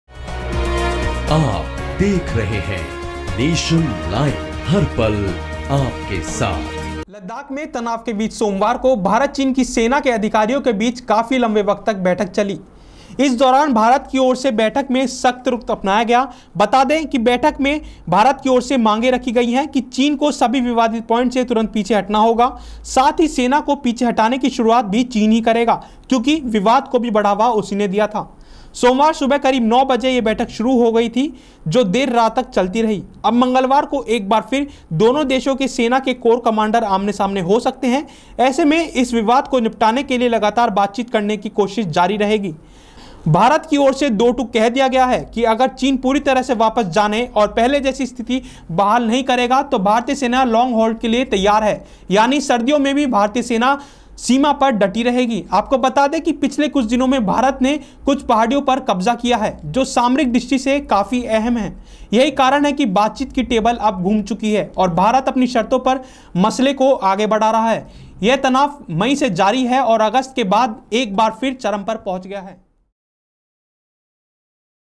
भारत-चीन के बीच मोल्डो में 13 घंटे तक चली कोर कमांडर-स्तरीय बैठक | India China border fight today
भारत-चीन के बीच मोल्डो में 13 घंटे तक चली कोर कमांडर-स्तरीय बैठक
चीन लद्दाख सीमा पर सभी विवादित प्वाइंट्स से हटे पीछे  चीन
चीन ने पहले नियम तोड़े थे, ऐसे में सैनिकों को पीछे हटाने की शुरुआत करे चीन
लाइन ऑफ एक्चुअल कंट्रोल से दोनों ओर से सेना को पीछे हटाने का रोडमैप तैयार

लद्दाख में तनाव के बीच सोमवार को भारत-चीन की सेना के अधिकारियों के बीच काफी लंबे वक्त तक बैठक चली... इस दौरान भारत की ओर से बैठक में सख्त रुख अपनाया गया. बैठक में भारत की ओर से मांग रखी गई है कि चीन को सभी विवादित प्वाइंट से तुरंत पीछे हटना चाहिए. साथ ही सेना को पीछे हटाने की शुरुआत चीन ही करे, क्योंकि विवाद को बढ़ावा भी उसने ही दिया है. सोमवार सुबह करीब नौ बजे ये बैठक शुरू हुई, जो देर रात तक चलती रही. अब मंगलवार को एक बार फिर दोनों देशों की सेना के कोर कमांडर आमने-सामने हो सकते हैं. ऐसे में इस विवाद को निपटाने के लिए लगातार बातचीत करने की कोशिश जारी रहेगी. भारत की ओर से दो टूक कह दिया गया है कि अगर चीन पूरी तरह से वापस जाने और पहले जैसी स्थिति बहाल नहीं करेगा, तो भारतीय सेना लॉन्ग हॉल के लिए तैयार है. यानी सर्दियों में भी भारतीय सेना सीमा पर डटी रहेगी. ...आपको बता दें कि पिछले कुछ दिनों में भारत ने कुछ पहाड़ियों पर कब्जा किया है, जो सामरिक दृष्टि से काफी अहम हैं. यही कारण है कि बातचीत की टेबल अब घूम चुकी है और भारत अपनी शर्तों पर मसले को आगे बढ़ा रहा है. ये तनाव मई से जारी है और अगस्त के बाद एक बार फिर चरम पर पहुंचा है.